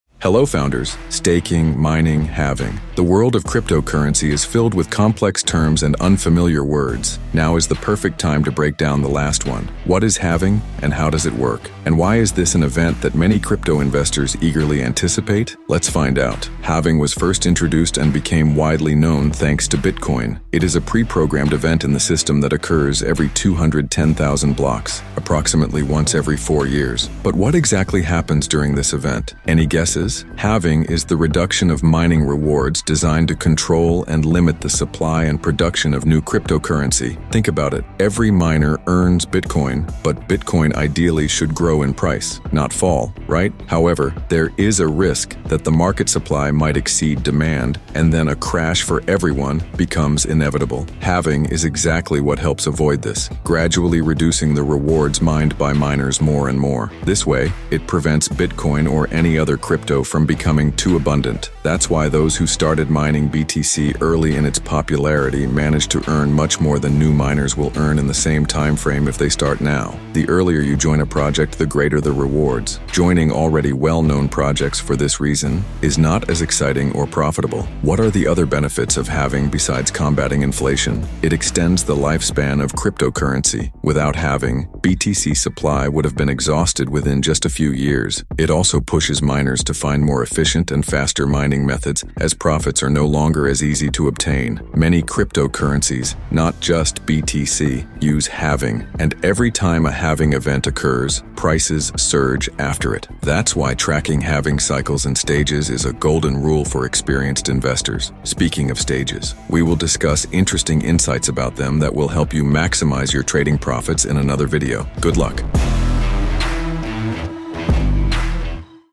What Is Halving and Why Do Crypto Investors Expect It?
📱 Play, win, and get rewards in Battle Bulls!

Hey, founders! Staking, mining, halving… The crypto world is full of complex concepts and confusing terms! Now is the perfect time to break down the last one—what is halving, and how does it work? Plus, why is it an event that crypto investors eagerly anticipate, rubbing their hands in excitement? We’ll explain everything in this video!